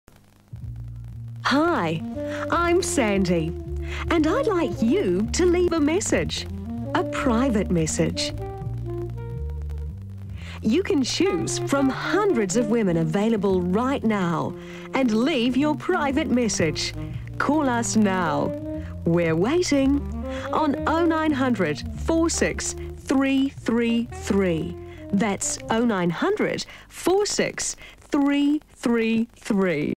"Heart to Heart Dateline" late night 90's TV ad
New Zealand late night dating hotline ad. Circa 1999.

Dubbed from a PAL VHS tape, de-interlaced and upscaled to 720p so YouTube preserves the full resolution and frame-rate.